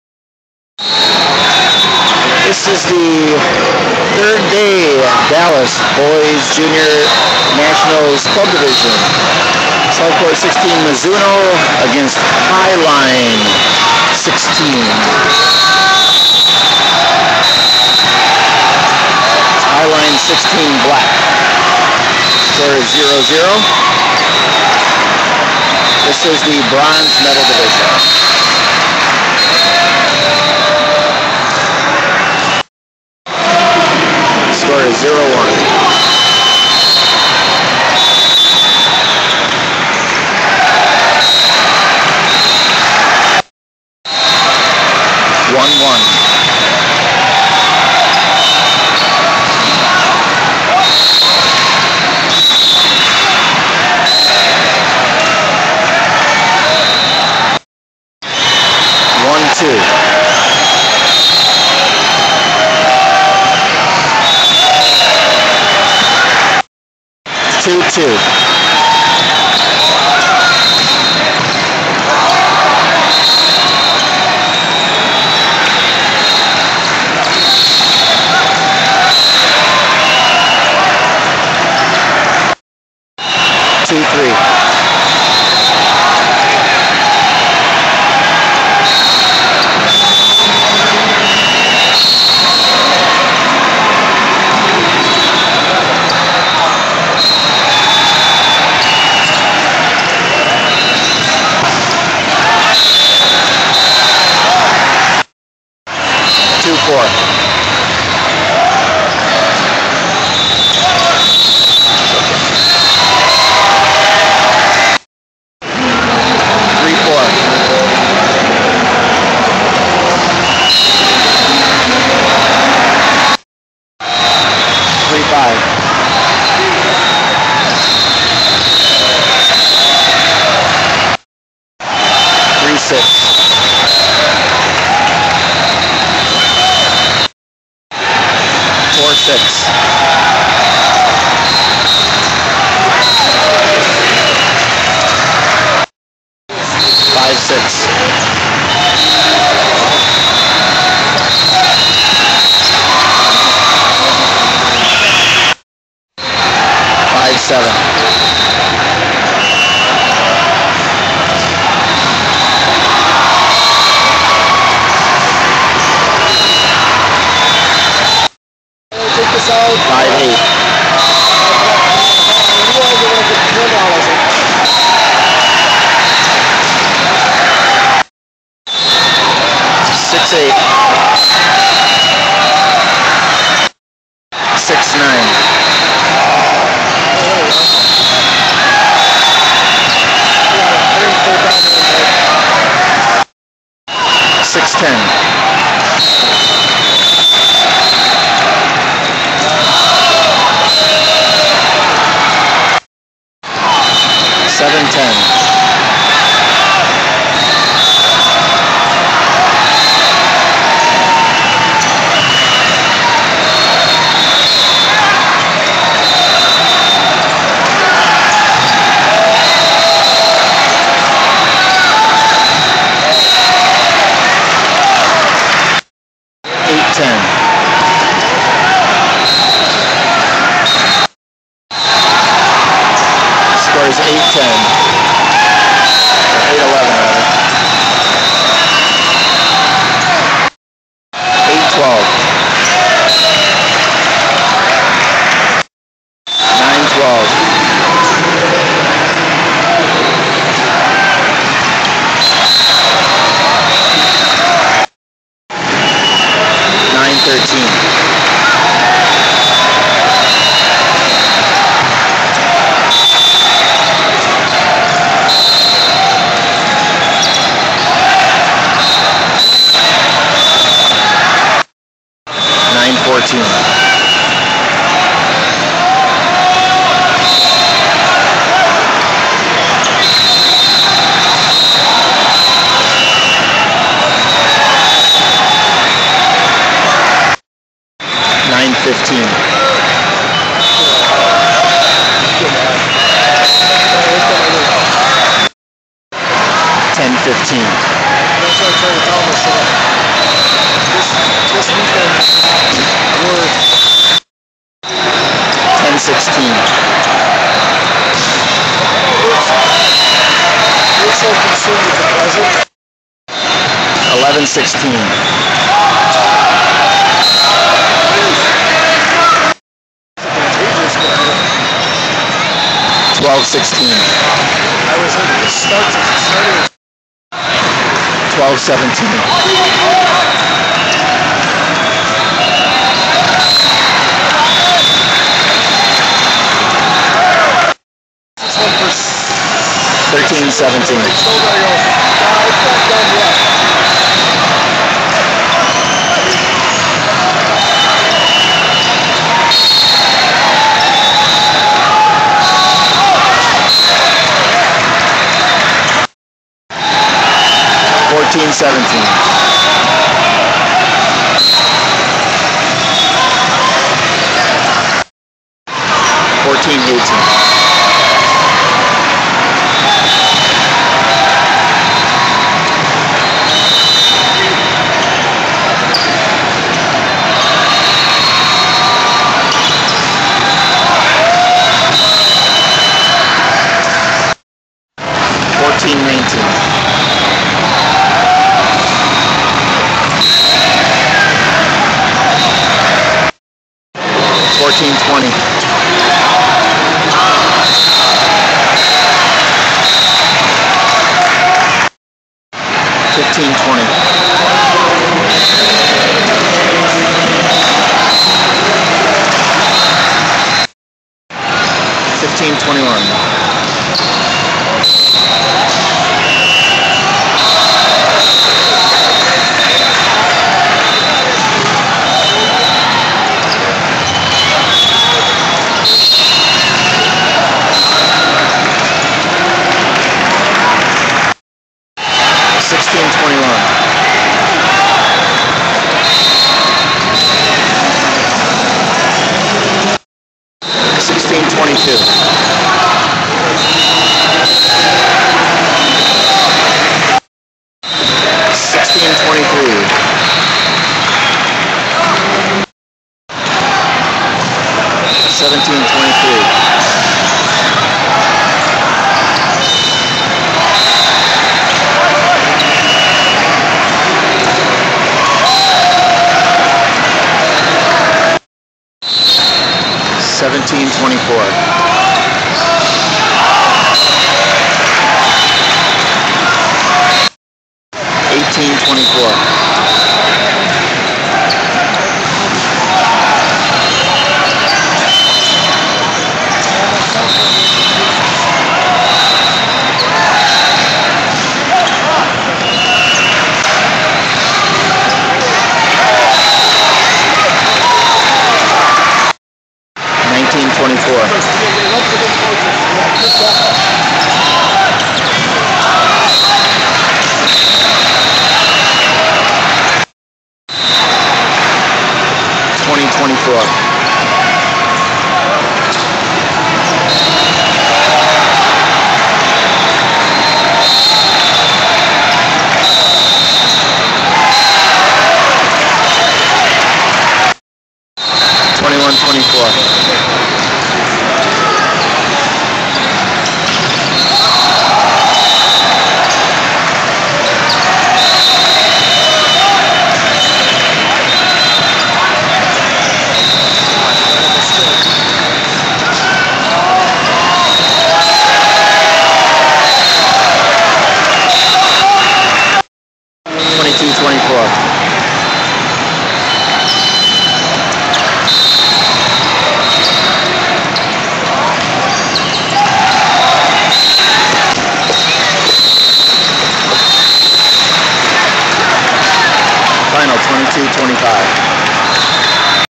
Southport 16 vs Highline 16 Black - Game 1 - Boys Volleyball - Nationals - Dallas
July 6, 2012: Southport 16 Mizuno vs. Highline 16 Black
BJNC Volleyball - Club Division - Dallas, TX
Final Score: Highline 25 - Southport 22